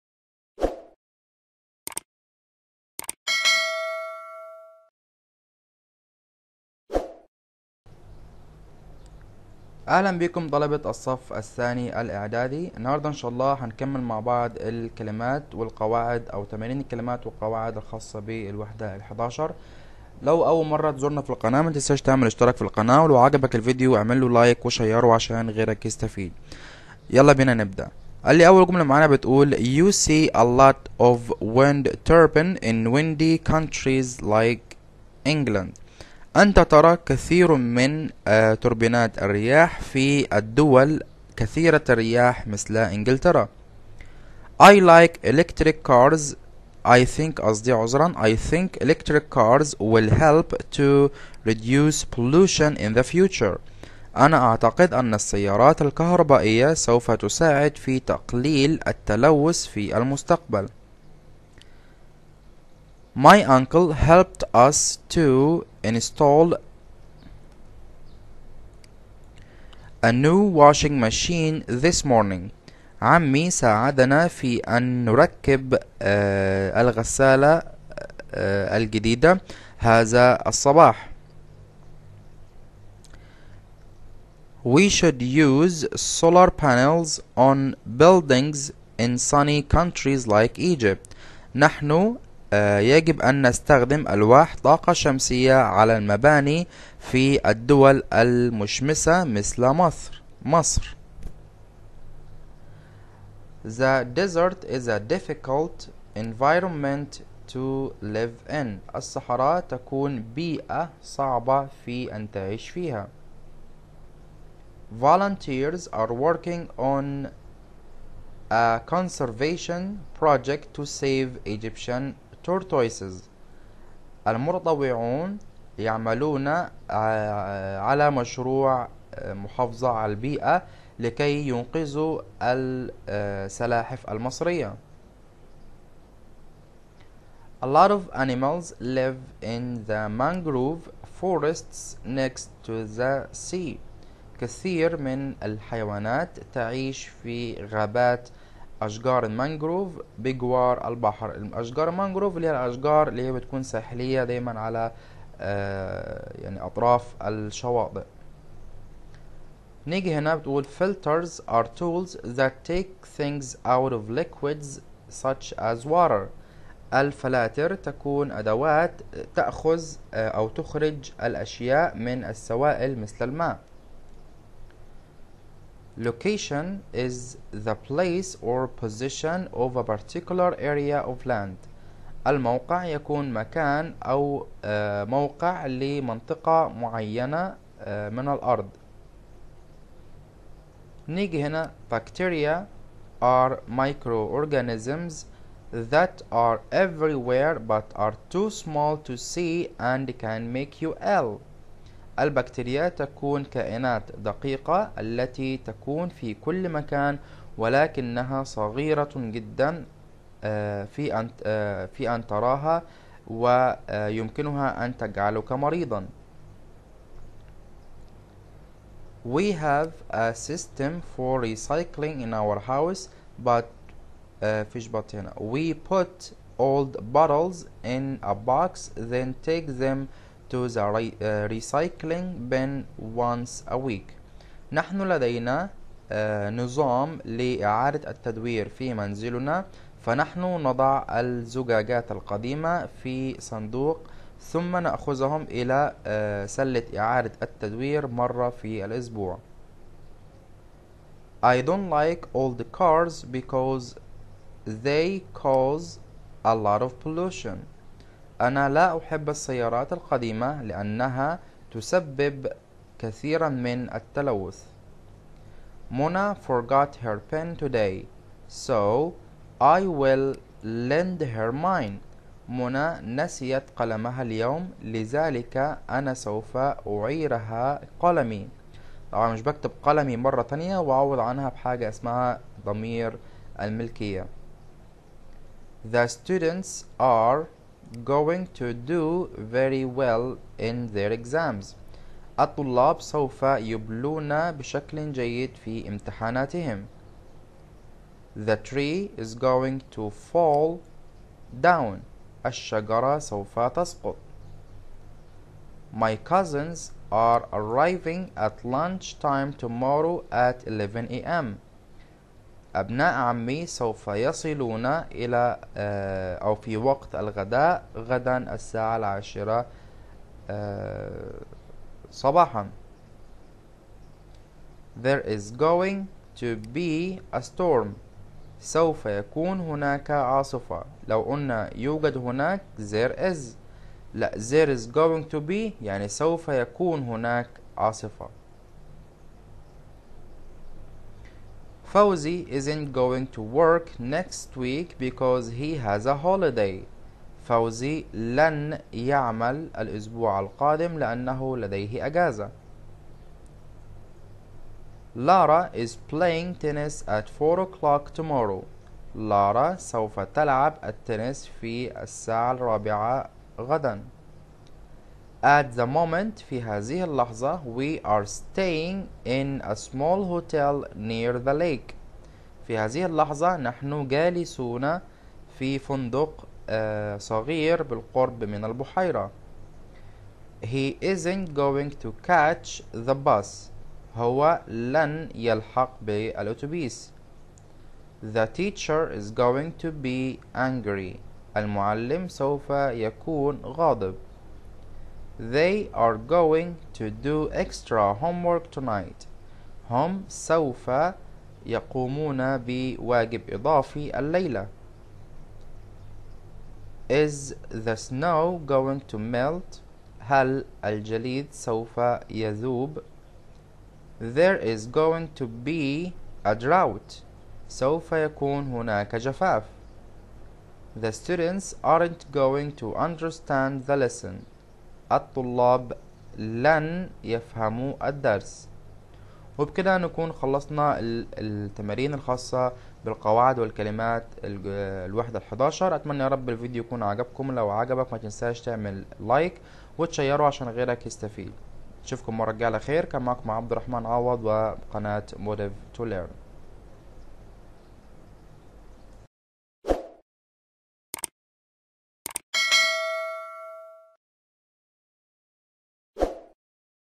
اللغة الانجليزية الصف الثاني الإعدادي الترم الثاني 2022 مراجعة وحل تمارين الوحدة الحادية عشر 11
اللغة_الانجليزية_الصف_الثاني_الإعدادي_الترم_الثاني_2022
حل تمارين ملحق كتاب جيم  ongoing assessment 2022